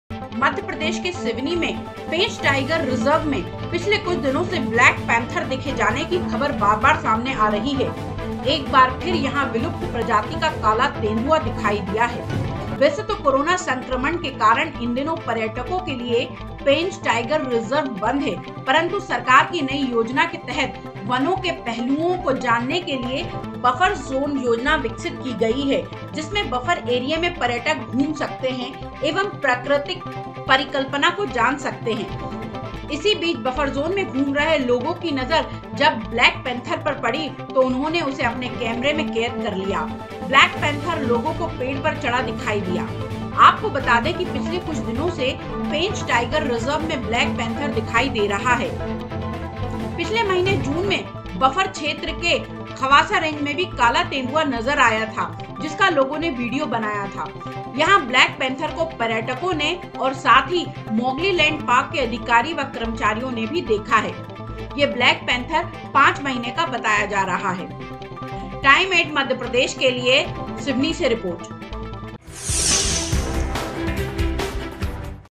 TIME8 GROUND REPORT I पेंच टाइगर रिज़र्व में दिखा दुलर्भ 'ब्लैक टाईगर'
सिवनी।  पेंच टाइगर रिज़र्व में पर्यटकों और ग्रामीणों को दिखा दुलर्भ 'ब्लैक टाईगर'। विलुप्त माना जा रहा था 'ब्लैक टाईगर'।